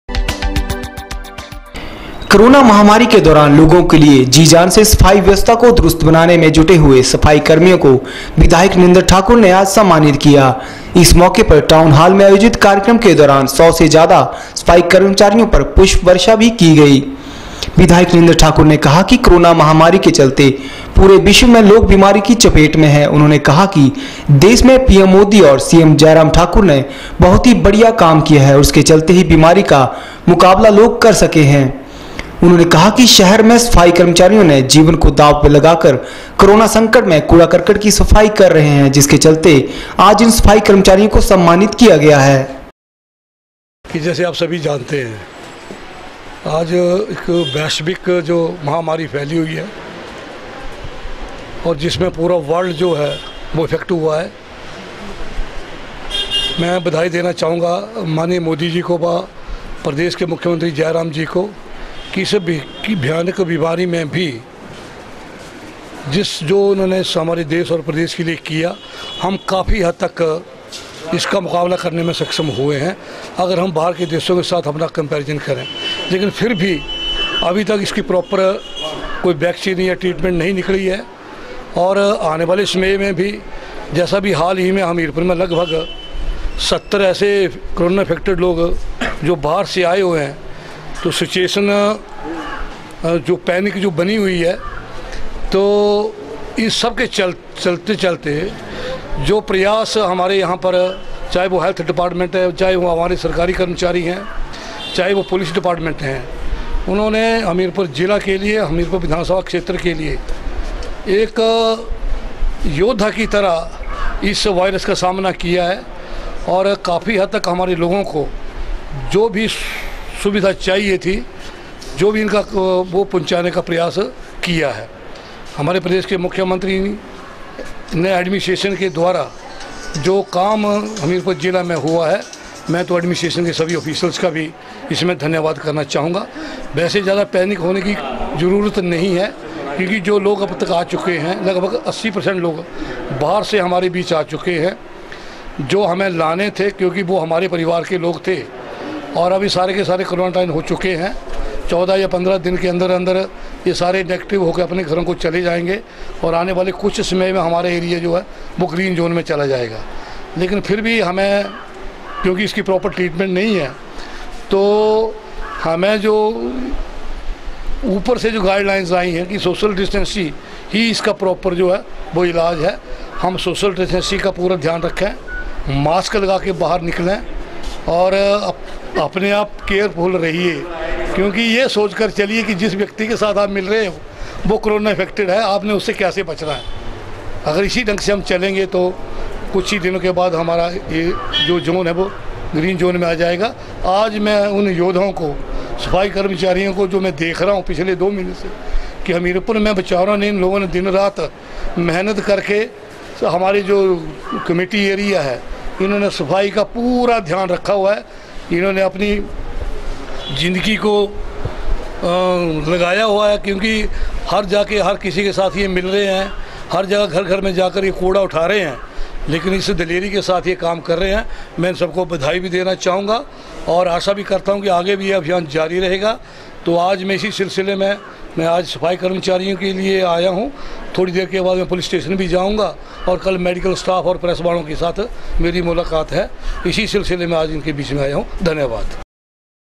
27 may 07 सफाई कर्मियों को विधायक नरेन्द्र ठाकुर ने सम्मानित किया
कोरोना माहमारी के दौरान लोगों के लिए जी जान से सफाई व्यवस्था को दुरूस्त बनाने में जुटे हुए सफाई कर्मियों को विधायक नरेन्द्र ठाकुर ने सम्मानित किया। इस अवसर पर टाउन हाल में आयोजित र्कार्यक्रम के दौरान सौ से ज्यादा सफाईकर्मियों पर पुष्प बर्षा कर सम्मानित किया गया।
विधायक नरेन्द्र ठाकुर ने कहा कि कोरोना माहमारी के चलते पूरे विश्व में लोग बीमारी की चपेट में है। उन्होंने कहा कि देश में पीएम मोदी और सीएम जयराम ठाकुर ने बहुत बढिया काम किया है और इसके चलते ही बीमारी का मुकाबला कर सके है। उन्होंने कहा कि शहर में सफाई कर्मियों ने जीवन को दाव परलगाकर कोरोना संकट में कूडा कर्कट की सफाई कर रहेह ै जिसके चलते आज इन्हें सम्मानित किया गया है।